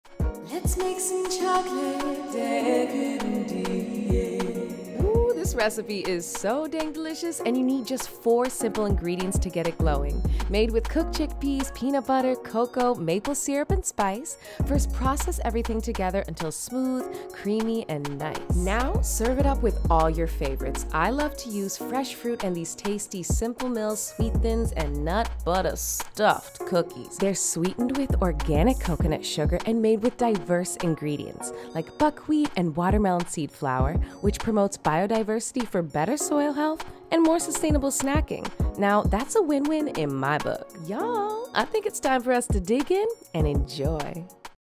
HOW TO MAKE THE ULTIMATE CHOCOLATE DESSERT!
CHOCOLATE DESSERT BOARD BY 🎵 @onegreatvegan 🍫 My Chocolate Decadent Dip is the perfect way to get your dessert board glowing! This recipe is so quick and easy to make, plus it only requires four simple ingredients: cooked chickpeas, peanut butter, cocoa, maple syrup, and a dash of spice. It’s truly delicious - giving you the perfect tasty treat for any occasion. Don’t forget to load it up with all of your favorite fruit, nuts, crackers, and cookies! 🍪 I love to use Simple Mills Stuffed Sandwich Cookies and Sweet Thins as the ultimate dipping device because all of their ingredients are wholesome, diverse, and made with sustainability in mind. I picked up these glorious goodies at Sprouts Farmers Market and now that you are officially drooling, it’s time to dive into this chocolatey decadence and enjoy! 🎵 @onegreatvegan